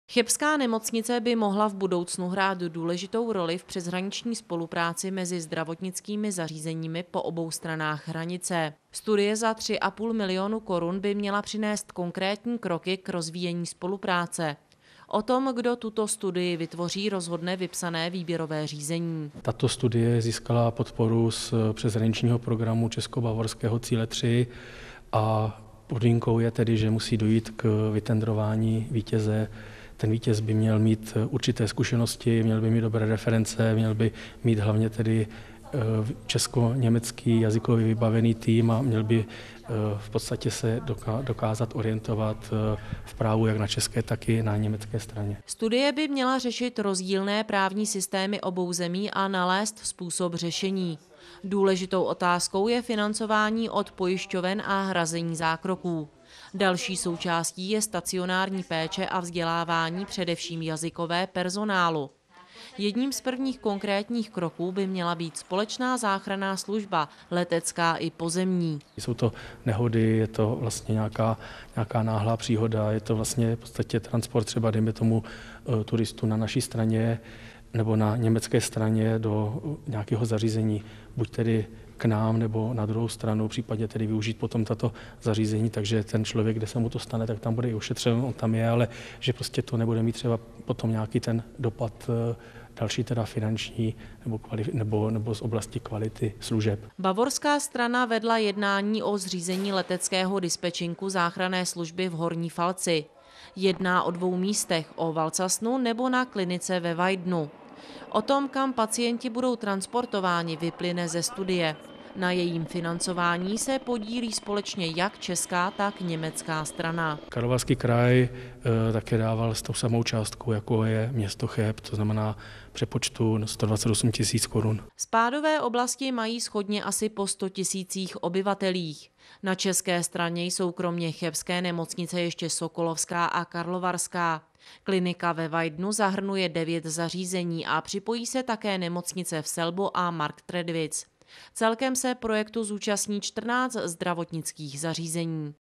2010 – Cheb: Přeshraniční spolupráci nemocnic popíše nová studie (3985) (TV Západ)
Chebská nemocnice by mohla v budoucnu hrát důležitou roli v přeshraniční spolupráci mezi zdravotnickými zařízeními po obou stranách hranice. Studie za 3,5 milionu by měla přinést konkrétní kroky k rozvíjení spolupráce. O tom, kdo tuto studii vytvoří, rozhodne vypsané výběrové řízení.

„Tato studie získala podporu z přeshraničního programu Česko-bavorského CÍL 3. Podmínkou je, že musí dojít k vytendrování vítěze. Ten by měl mít určité zkušenosti, dobré reference a mít hlavně Česko-německý jazykově vybavený tým a měl by se orientovat v právu na české i německé straně,“ vyjmenoval hlavní požadavky místostarosta Michal Pospíšil.

Studie by měla řešit rozdílné právní systémy obou zemí a nalézt způsob řešení. Důležitou otázkou je financování od pojišťoven a hrazení zákroků. Další součástí je stacionární péče a vzdělávání, především jazykové, personálu. Jedním z prvních konkrétních kroků by měla být společná záchranná služba – letecká i pozemní.

„Jsou to nehody, náhlé příhody nebo transport turistů z obou stran do zařízení buď k nám, nebo na druhou stranu, případně využít toto zařízení, takže ten člověk, kde se mu to stane, bude i ošetřen a nebude to mít dopad finanční nebo v oblasti kvality služeb,“ uvedl příklady Pospíšil.

Bavorská strana vedla jednání o zřízení leteckého dispečinku záchranné služby v Horní Falci. Jedná o dvou místech, o Waldsassenu nebo na klinice ve Weidenu. O tom, kam pacienti budou transportováni vyplyne ze studie. Na jejím financování se podílí společně jak česká, tak německá strana.

„Karlovarský kraj také dával tu samou částku jako město Cheb, to znamená v přepočtu 128 tisíc korun,“ připomněl Pospíšil.

Spádové oblasti mají shodně asi po 100 tisících obyvatelích. Na české straně jsou kromě chebské nemocnice ještě sokolovská a karlovarská. Klinika ve Weidenu zahrnuje devět zařízení a připojí se také nemocnice v Selbu a Marktredwitz. Celkem se projektu účastní 14 zdravotnických zařízení.